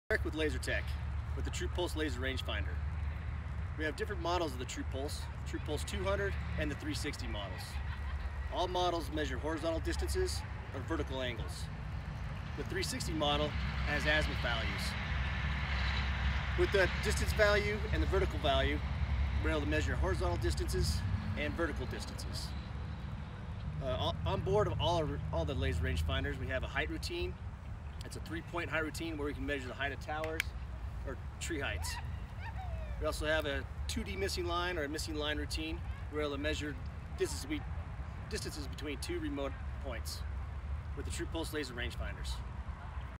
Discover: TruPulse Laser Rangefinders Models
Explore and analyze the differences of each TruPulse laser rangefinder model and uncover the calculations and routines you can produce when in the field

Explore the specs, key features, and field applications for our current TruPulse series professional-grade laser rangefinders